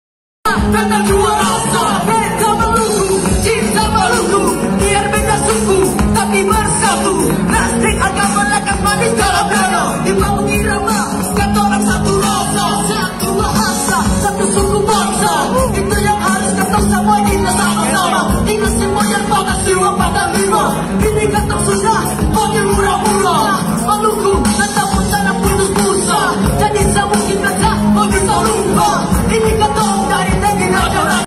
Ambon Kota[K] Musik
Ambon Kota(K) Musik · Live Performance Felish CHC

Kasbangga Maluku feat. Psycho Band

🔴 CHC Only One Live at 2nd Anniversary Ambon City of Music